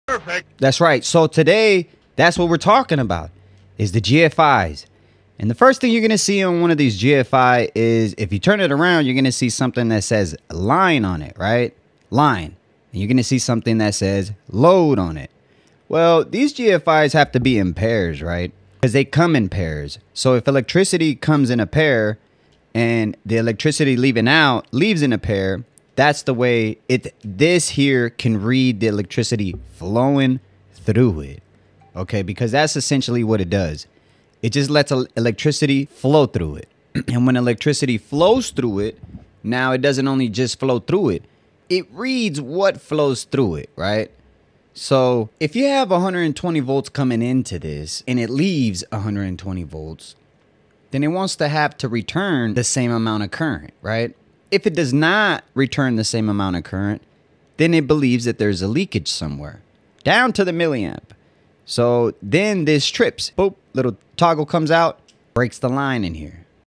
GFCI Breakers Explained 🔒 What They Do vs Regular Breakers EL Electrician
GFCI breakers are one of the most important safety devices in your home’s electrical system. But how are they different from a regular breaker? ⚡

In this video, I break down:
✅ What a regular breaker does (protects wiring from overloads & fire)
✅ What a GFCI breaker does (protects people from shock)
✅ How GFCIs sense current imbalances
✅ Where GFCIs are required in the home (bathrooms, kitchens, outdoors)
✅ GFCI breaker vs GFCI receptacle explained

Stay safe, protect your home, and understand your electrical system like a pro with EL Electrician.